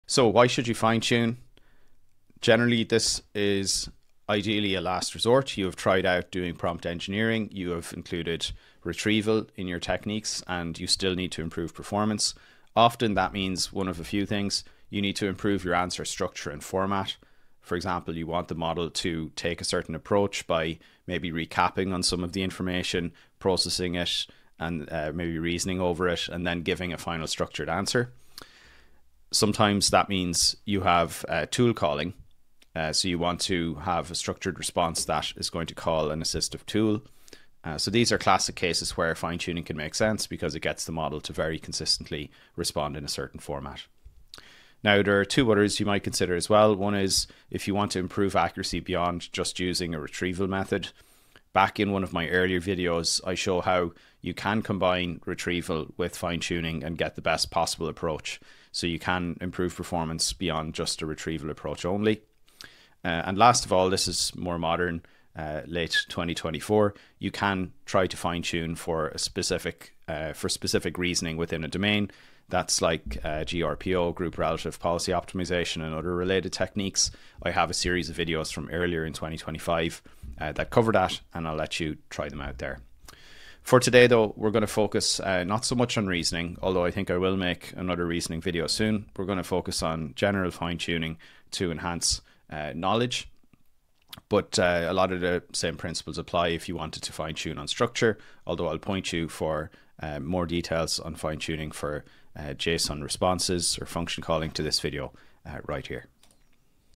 Why fine tune
📜Get repo access at Trelis.com/ADVANCED-fine-tuning

🤝 Are You a Top Developer?

💸 Starting a New Project/Venture?

TIMESTAMPS:
0:00 Fine-tune Gemma Llama Qwen Phi Mistral
0:27 Video Overview
1:35 When to do fine-tuning?
3:22 How to prepare a fine-tuning dataset?
6:24 Choosing unsloth versus transformers
11:07 How to run fast evaluations with vLLM
12:33 What model to fine-tune?
14:27 General fine-tuning tips
16:04 Fine-tuning notebooks and setup
19:11 Running fast evaluations with vLLM in a jupyter notebook
37:56 Fine-tuning with Unsloth
1:09:02 Fine-tuning with transformers
1:20:24 Conclusion